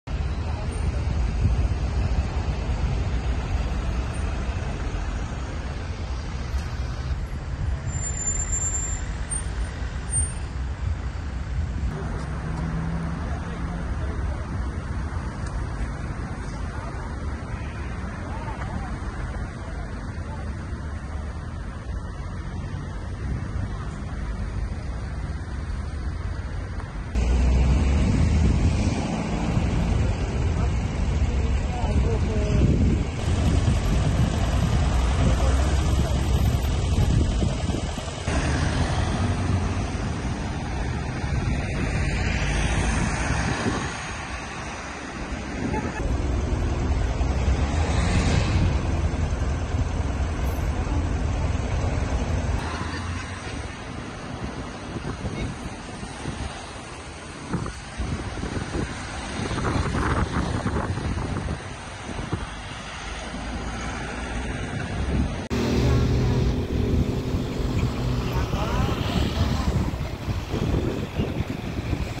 Apelul la 112 care a pus pe jar pompierii si politistii la Timisoara
‘Veniti repede, ca se intampla o nenorocire la etajul 10’. Apelul telefonic care a pus pe jar pompierii si politistii la Timisoara. Video